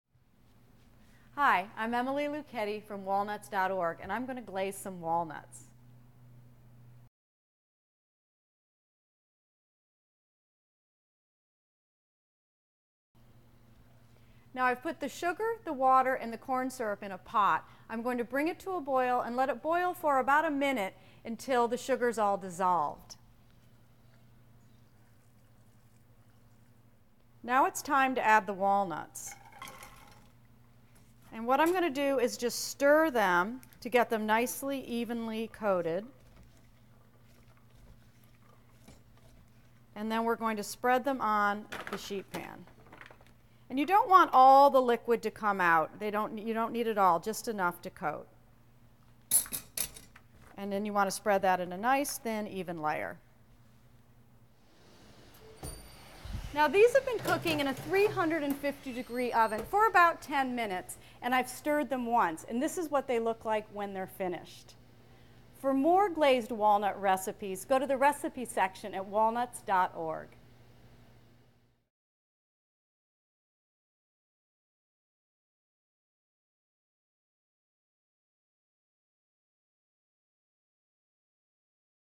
Glazed Walnuts with Emily Luchetti
Emily shows us how to glaze walnuts - a perfect addition for any holiday treat!